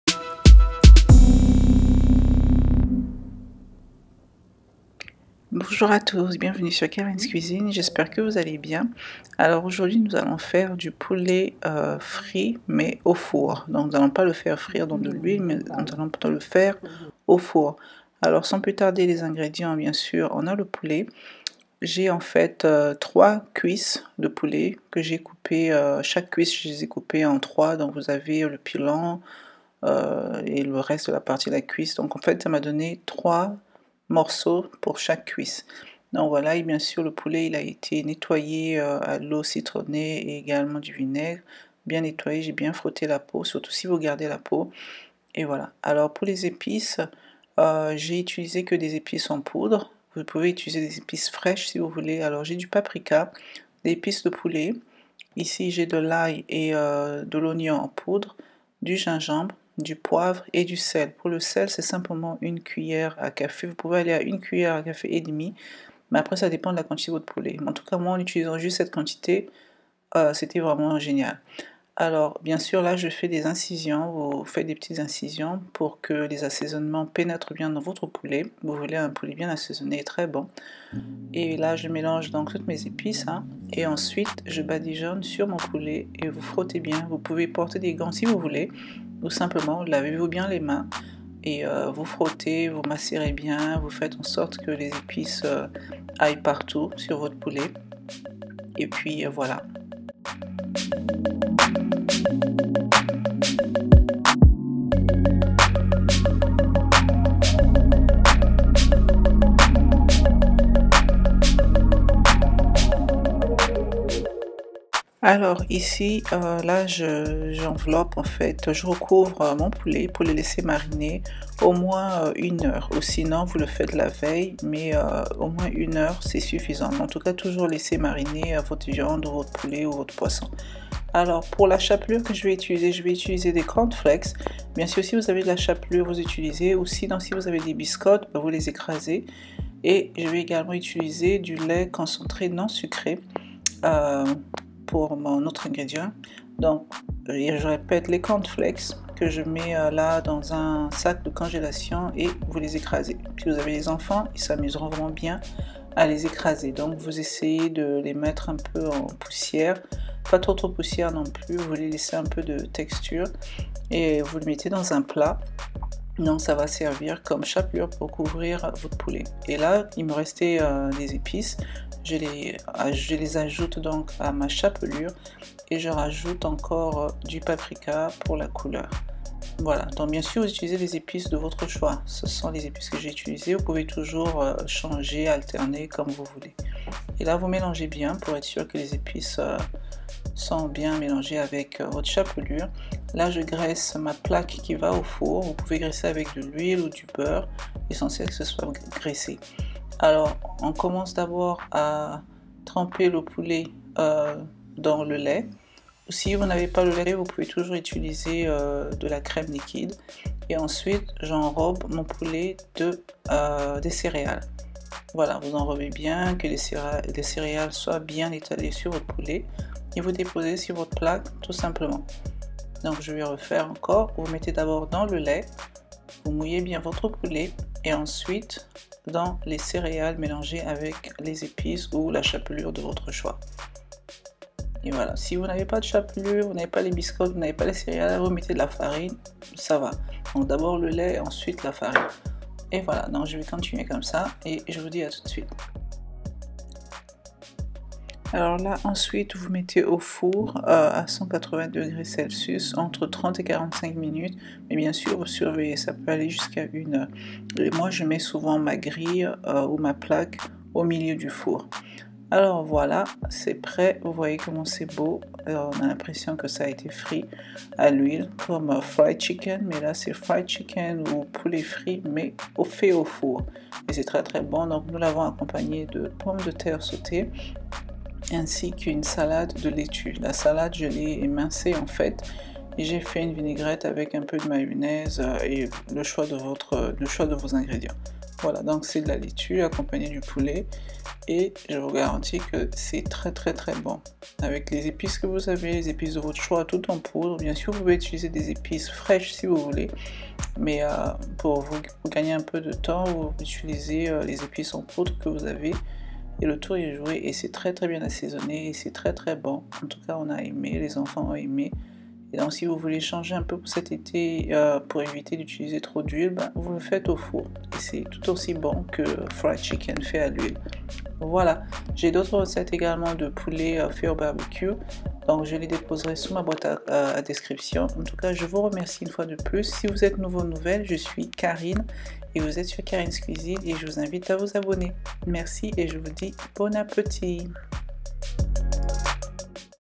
Fried chicken au Four/Poulet Frit au Four
Hello à tous, je vous montre comment je fais mon poulet frit mais au four (fried chicken au four). Très simple à faire, vous allez aimé.
Bonne appétit!

Ingrédients:                                                    Pour la chapelure
-3 cuisses de poulet                                      - Corn flakes(cereals)
-Paprika                                                           - Lait concentré non sucré
-Épice de poulet
-Oignon en poudre                                        Au four à 180°C-350°F (30-45 min ou plus)
-Gingembre en poudre
-Poivre en poudre
-Sel